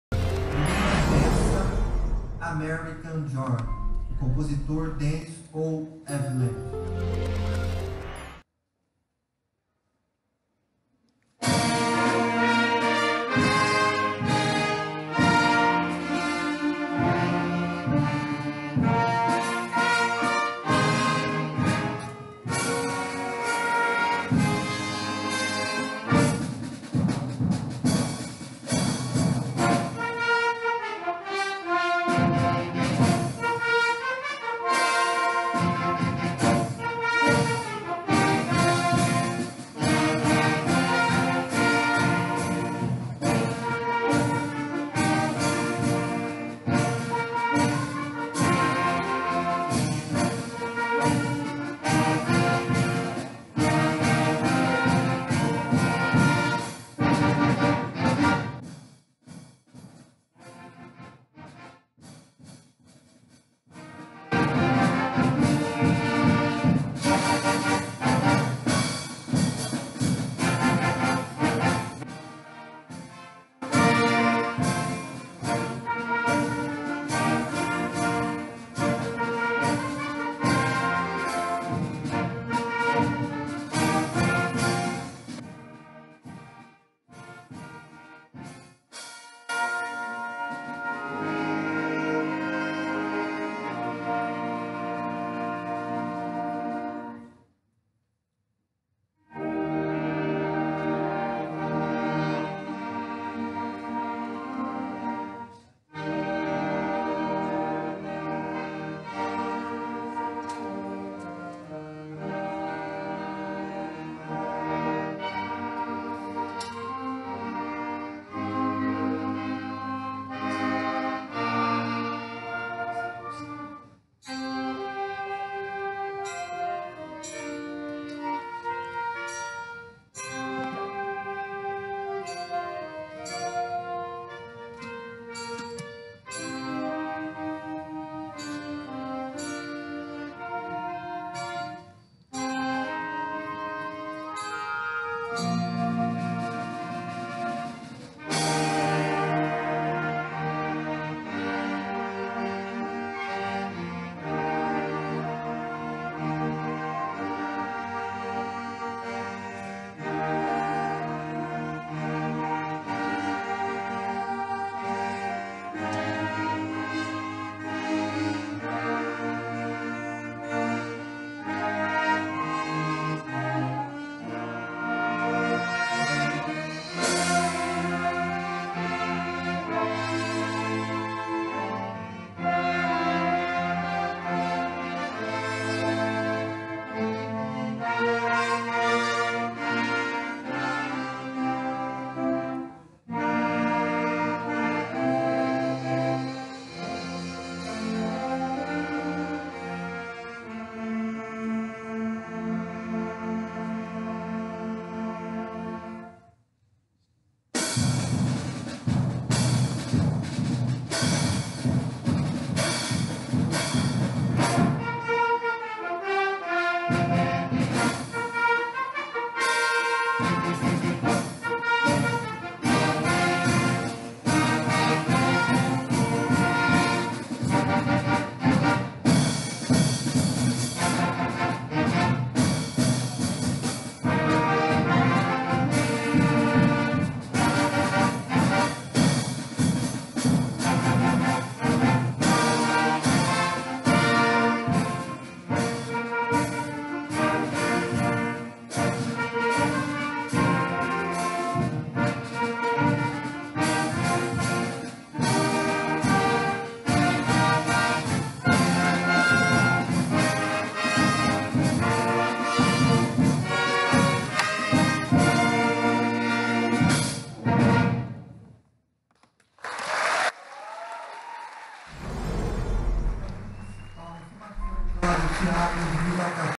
OCIFABAN - BANDAS MUSICAIS - CAIEIRAS 2018
Transmissão OFF-LINE da etapa de bandas musicais e sinfônicas, realizada no município de Caieiras - SP, pela Ocifaban.

Produção: PDM